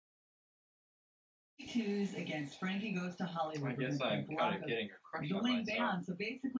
I think I"m getting a crush on myself
Self Explanatory. Prepping for a trip of a lifetime. Decided to "take a vacation" to Paris. One that I will never come back from. This is prepping. Haha.